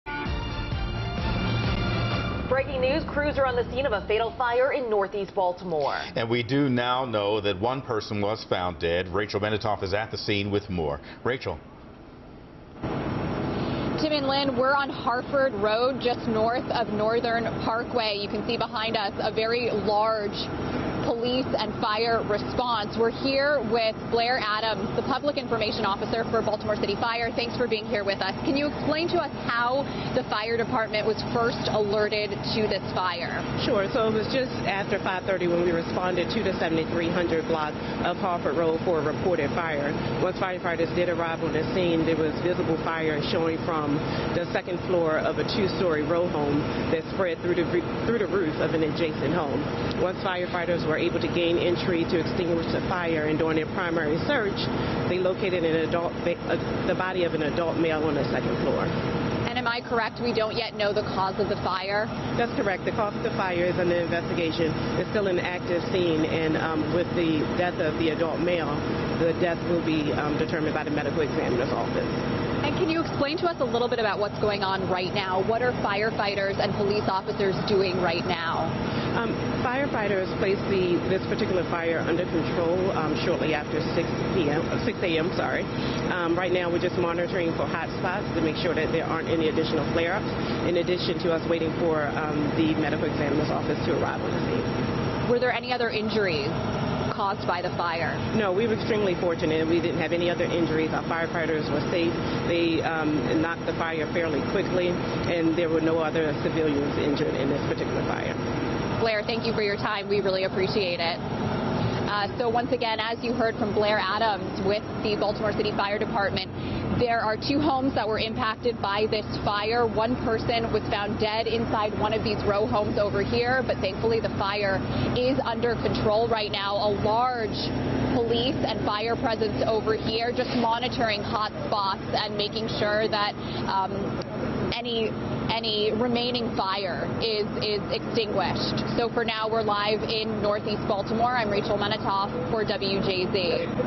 Fatal Fire Under Investigation In Northeast Baltimore
A person was found dead at a house fire in northeast Baltimore City.